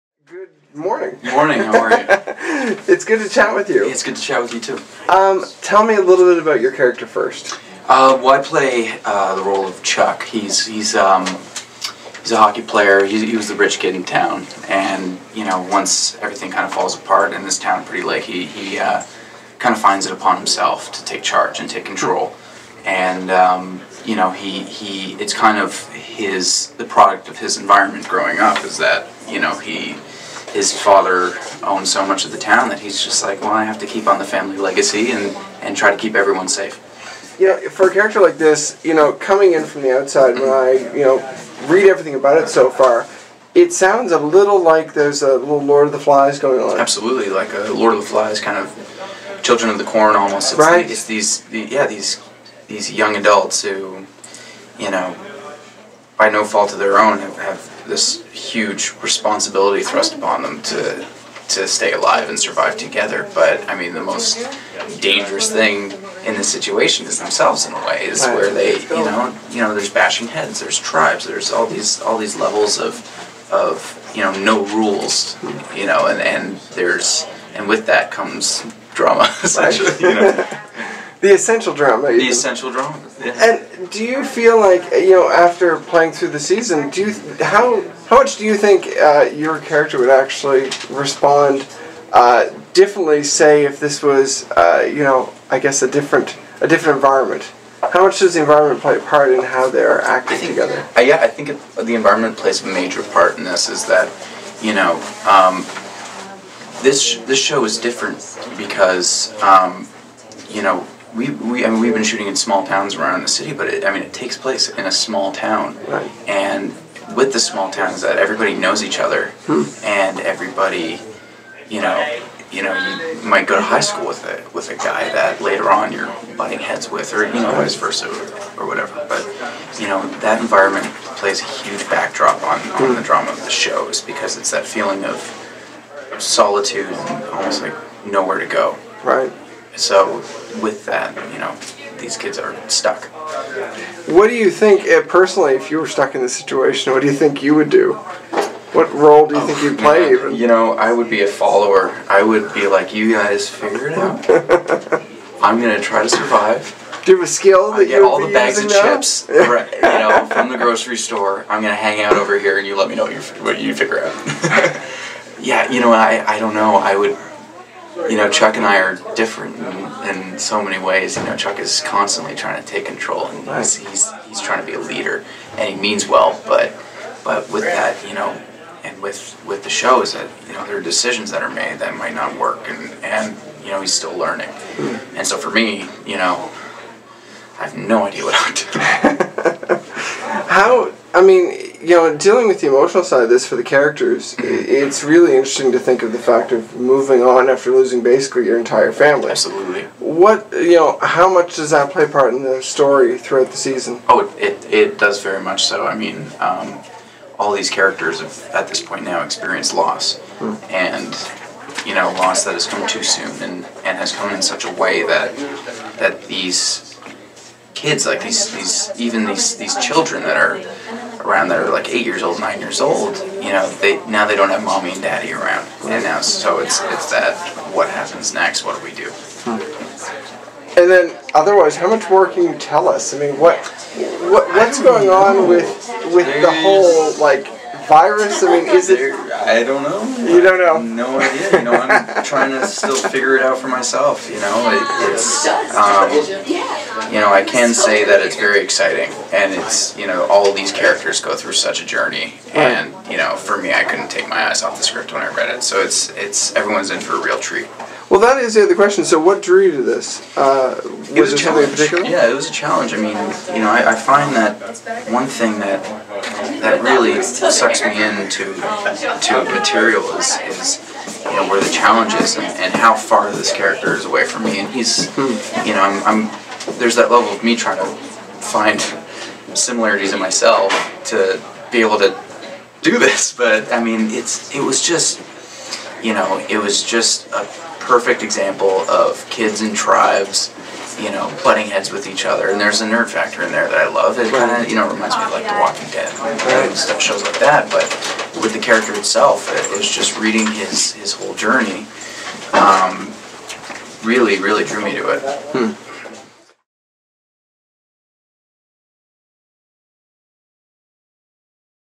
Justin Kelly on Between
Actor Justin Kelly talks about his character Chuck on set of the first season of Between.